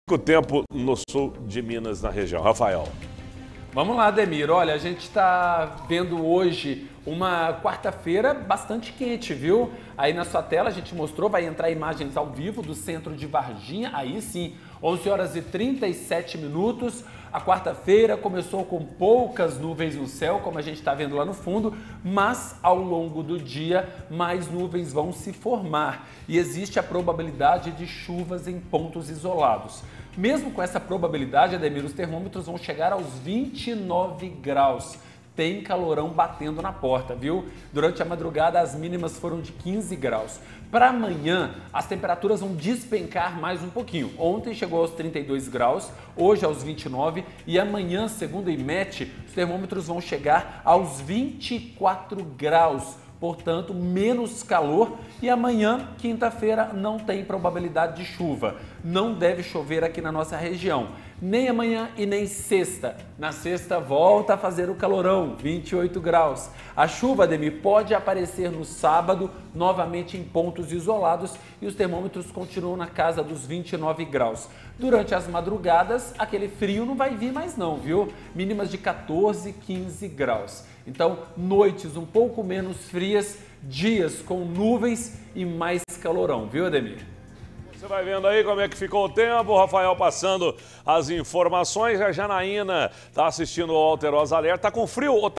Veja como fica o tempo na região
VEJA COMO FICO O TEMPO | A nossa equipe atualizou como fica o tempo na região.